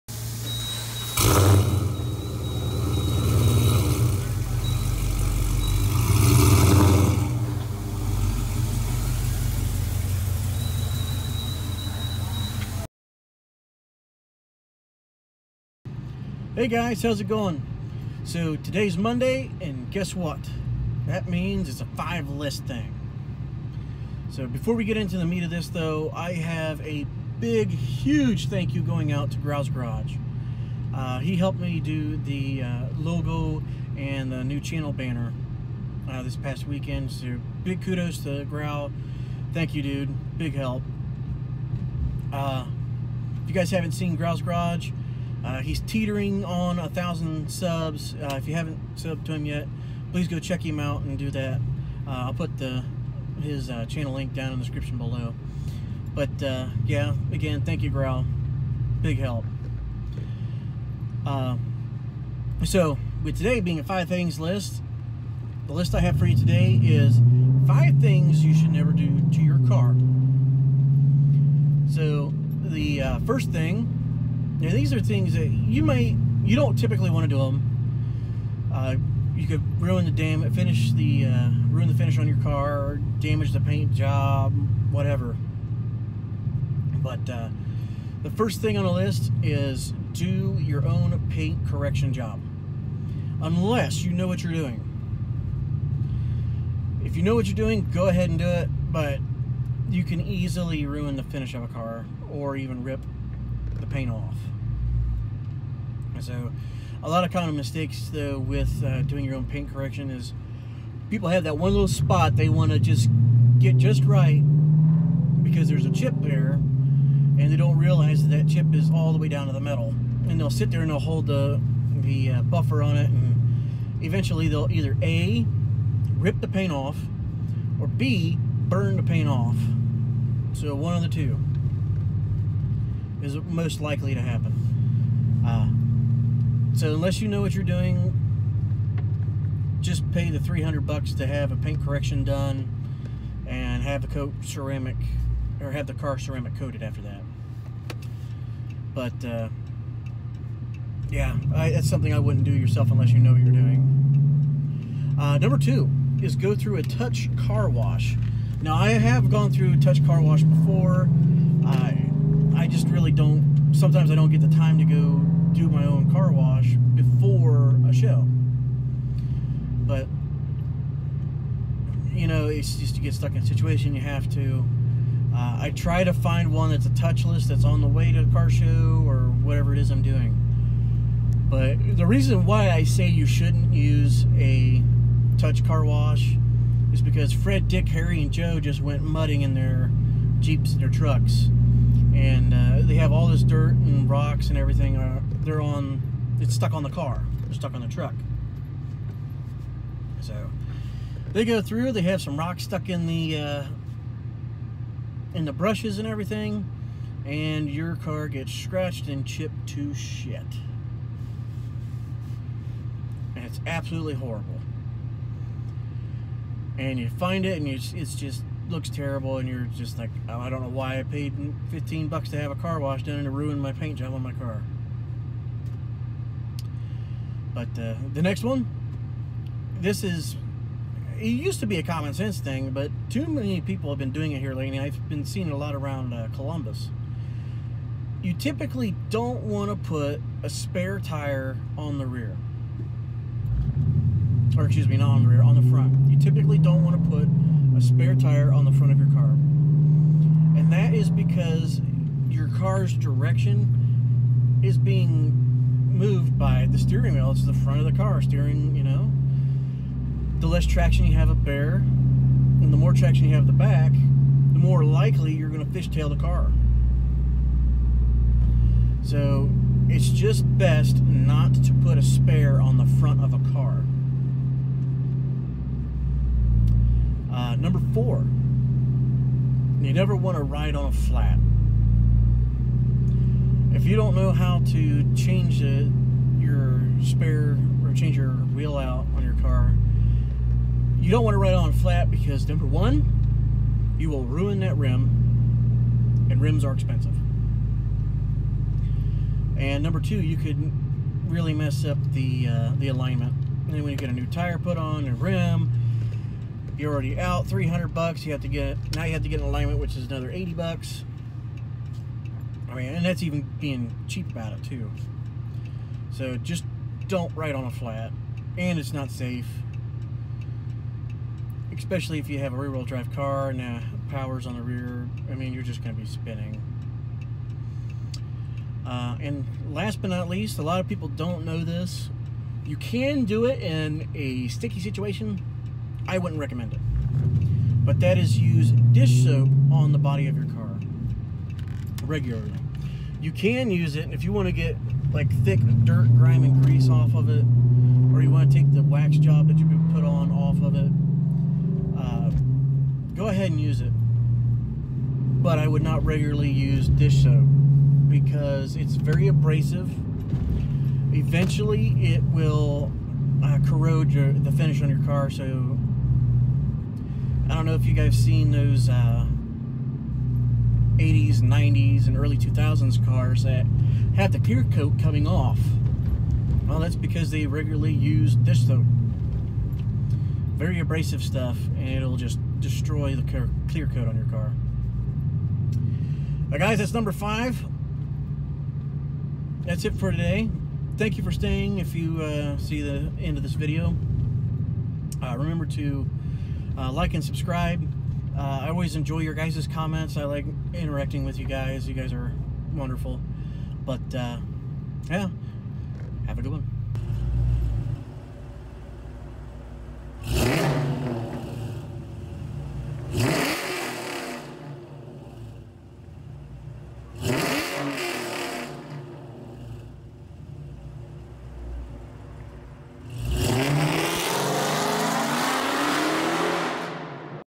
5 things to NEVER do to you car
Here is 5 things you should never do to your car! Big thank you to GrowlsGarage, please check him out if you haven’t yet.